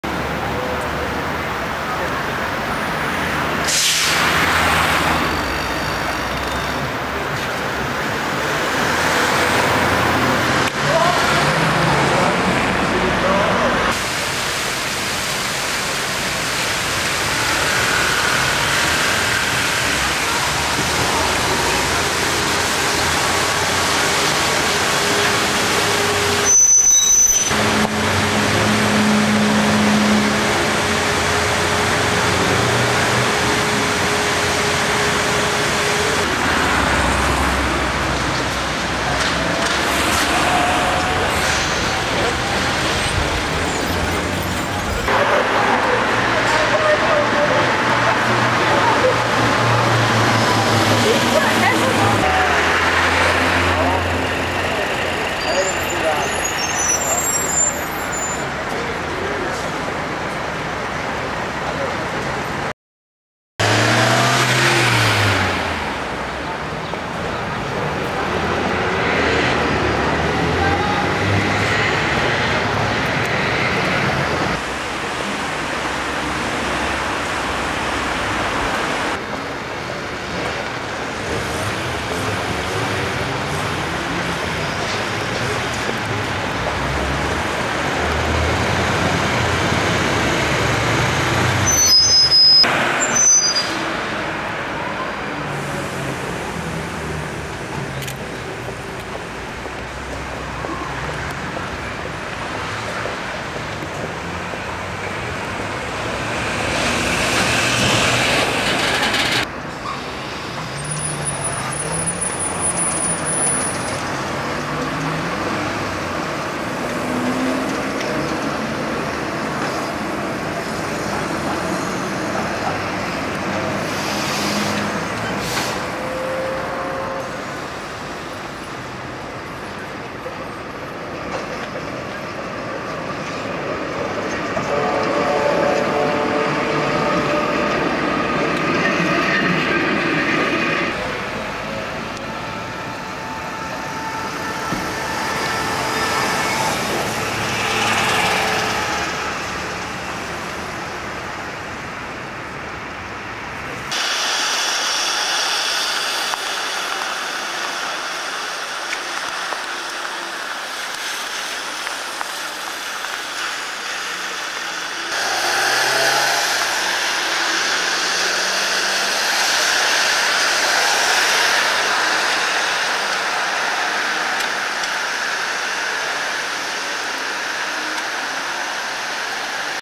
PMP DVD 1462 SWITZERLAND 1991 - 1992
A trailer for PMP DVD 1462 featuring trams, trolleys and buses in Switzerland in 1991 / 1992. Incldes Lugano, Spring 1991 then December 1992 visiting Zurich, Neuchatel and Biel. One of around 1500 transport titles by PMP
full list, links to blog and e bay shop  all via the web site. Titles £16 each post free world wide.